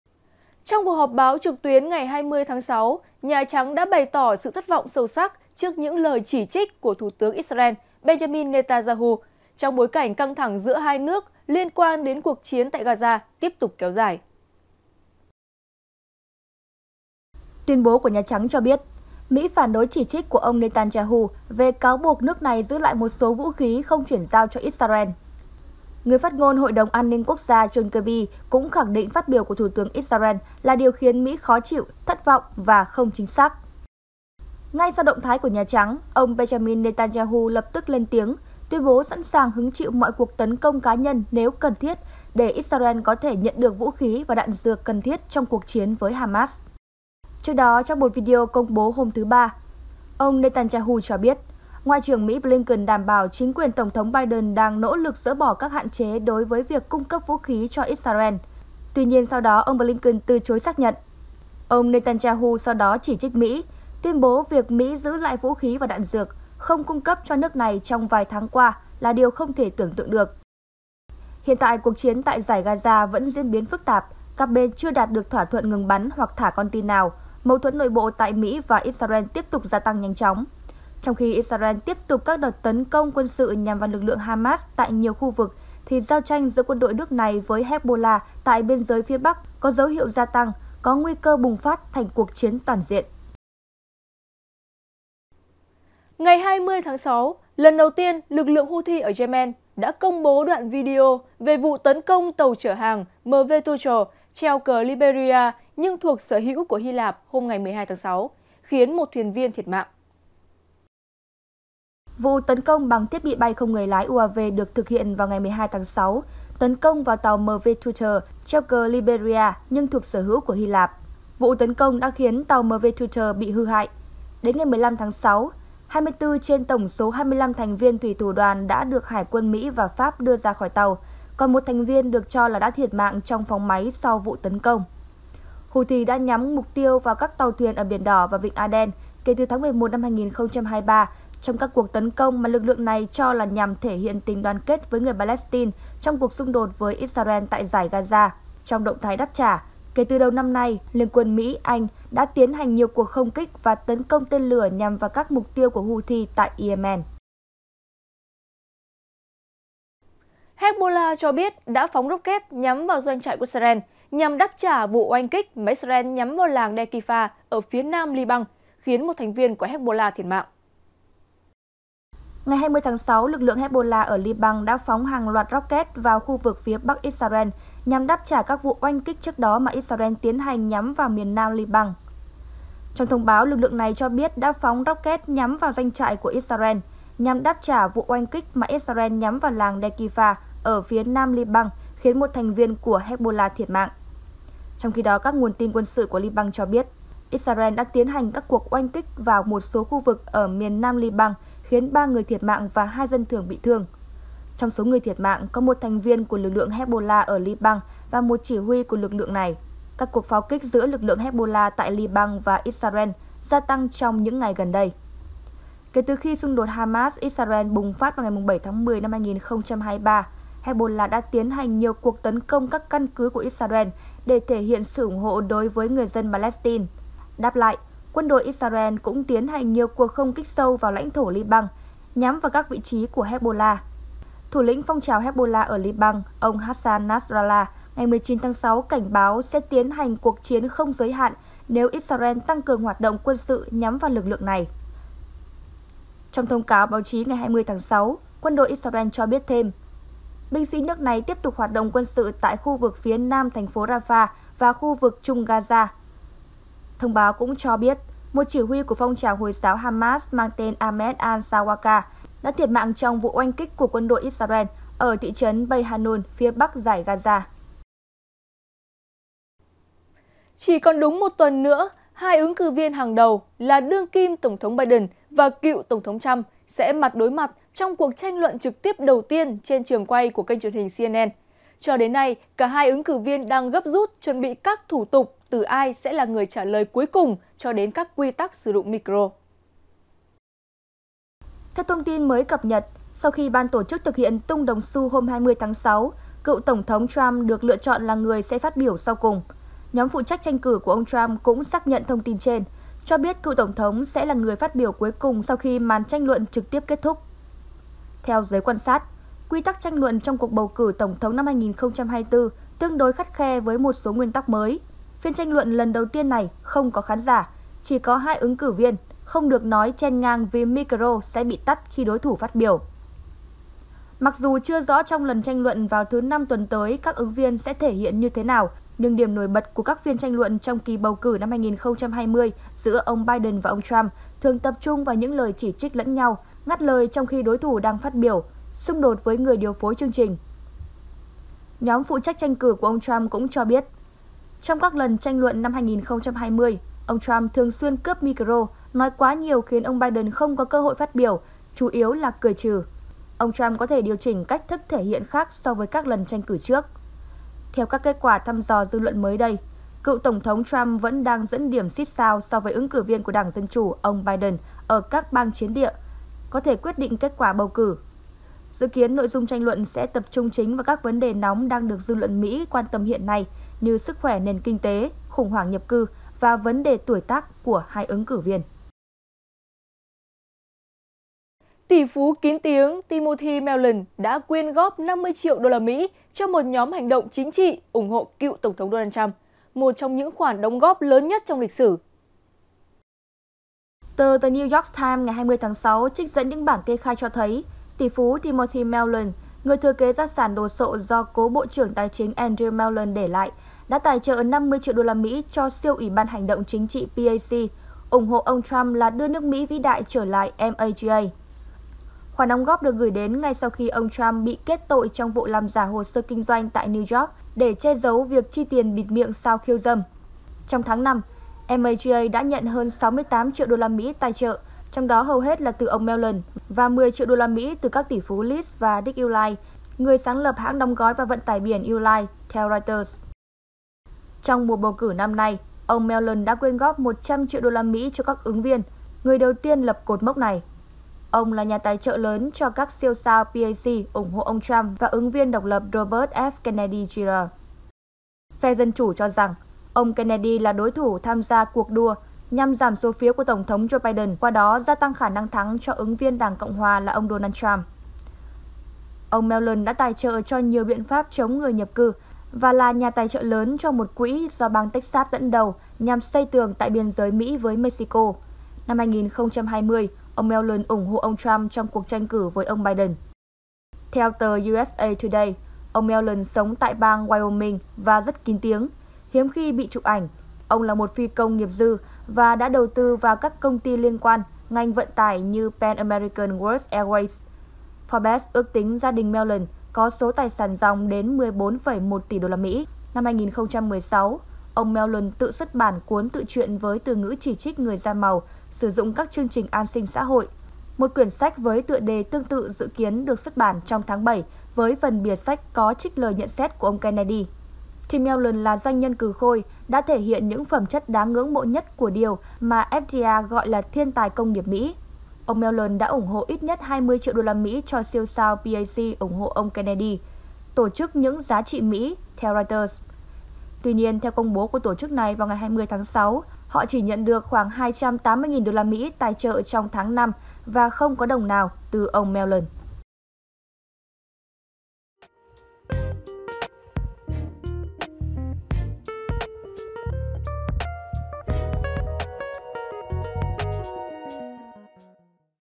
Mỹ - Israel tiếp tục khẩu chiến gay gắt về cuộc chiến Gaza | Báo Điện tử VOV
Báo Điện tử VOV | Trong cuộc họp báo trực tuyến ngày 20/6, Nhà Trắng đã bày tỏ sự thất vọng sâu sắc trước những lời chỉ trích của Thủ tướng Israel - Benjamin Netanyahu trong bối cảnh căng thẳng giữa hai nước liên quan đến cuộc chiến tại Gaza tiếp tục kéo dài.

37 Bà Triệu, Hoàn Kiếm, Hà Nội, Việt Nam